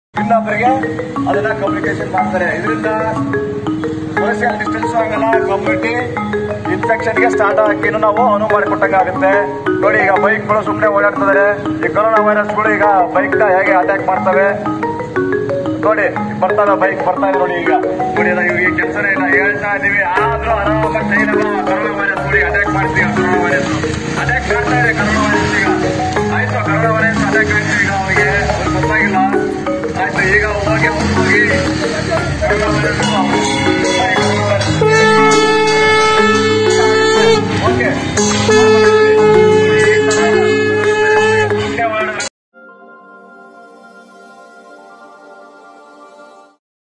Lockdown 21: Bengaluru cops use ‘corona helmets’ to raise awareness
To cope with motorists coming out on the streets during the ongoing 21-day lockdown, Bengaluru traffic cops have come up with ‘corona helmets’ to raise awareness on the importance of social distancing and isolation. Traffic cops have fashioned their headgears to look like a representation of coronavirus. Cops stop motorists and appeal to them to stop venturing out unnecessarily.